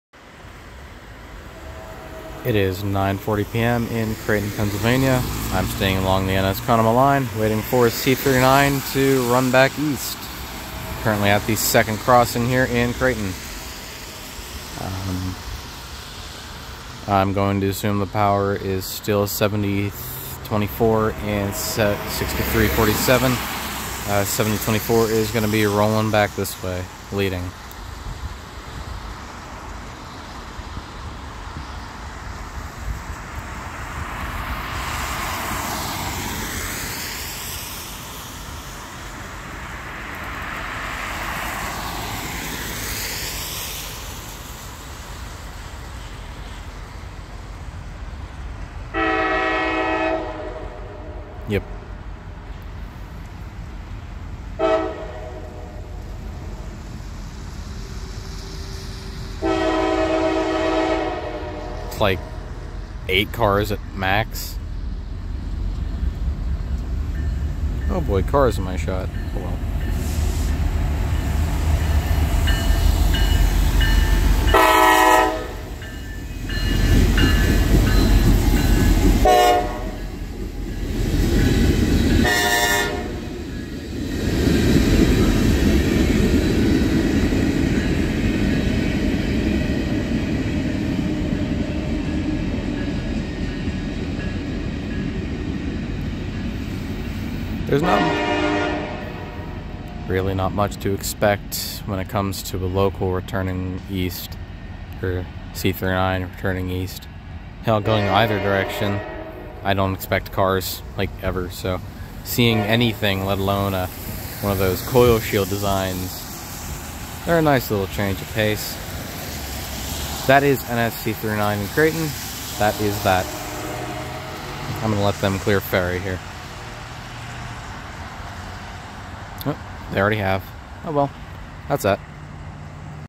NS 7024 Leads C39 East Through Creighton, PA | 5-4-24
Filming started at 9:40pm